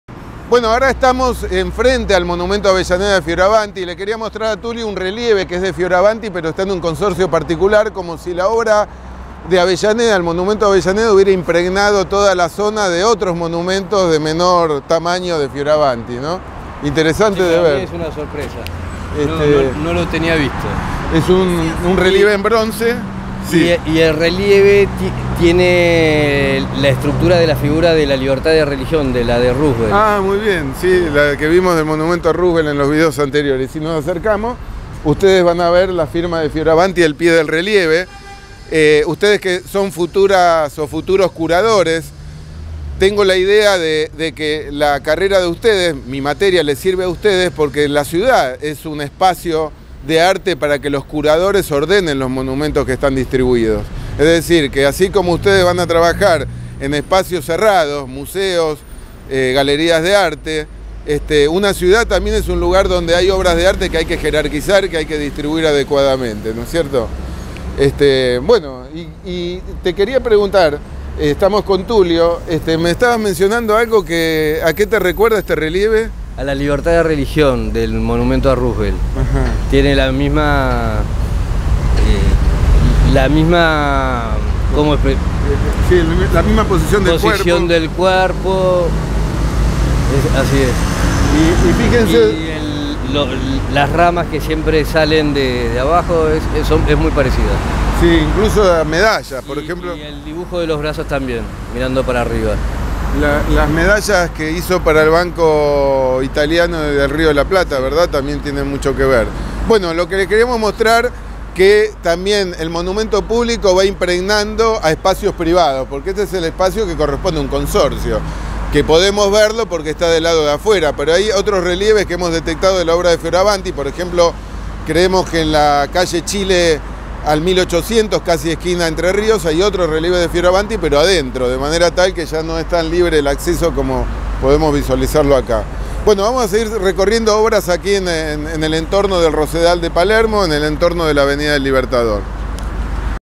10 DESCUBRIENDO A UN FIORAVANTI PRIVADO JUNTO A TULIO ANDREUSSI Y JUAN LAZARA ESEADE
Clase pública del Dr. Juan Antonio Lázara para sus alumnos de la asignatura de Patrimonio Público II de la Licenciatura en Curaduría de Arte del Instituto Universitario ESEADE con la participación del coleccionista Tulio Andreussi y de Javier Hermida Community Manager de redes sociales relacionadas con monumentos arquitectónicos y escultóricos de Buenos Aires. El recorrido se realizó por Palermo, visitando el Monumento a Garibaldi y luego visitando otros monumentos de José Fioravanti como los dedicados a Roosevelt, a Avellaneda y a Shakespeare. También se visitaron importantes y casi desconocidas obras del Rosedal como Flor de Juventud de Pedro Zonza Briano, el monumento a Lucio Correa Morales y el dedicado a Amado Nervo de Alberto Lagos. También se rindió un homenaje a la historiadora del arte Sonia Berjman especialista en espacios verdes. La recorrida incluyó el Monumento a Sarmiento de Rodin, el Monumento de los Españoles  y tuvo un histórico cierre con la visita a la oficina donde se restauran los monumentos de Buenos Aires MOA donde se pudieron ver en vivo y en directo obras de Bourdelle, Arturo Dresco y varios franceses del siglo XIX.